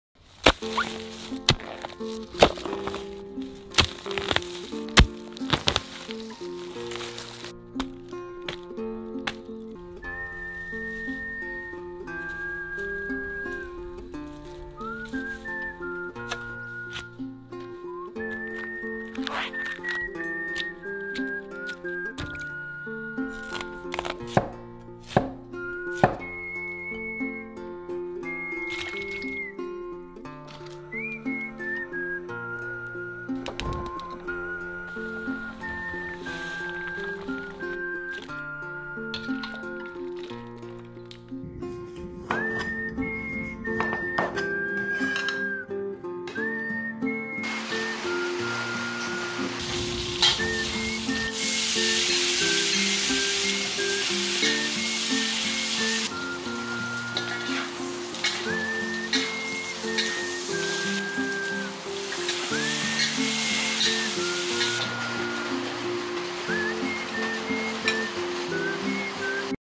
Fried smoke-cured pork with spring bamboo shoots
Following the spring rains, bamboo shoots can be found growing all over the mountains. For locals in Wufeng, fried smoke-cured pork with spring bamboo shoots is a must-have delicacy only available in this season.
Video by Chen Jingjing, Xiang Bo.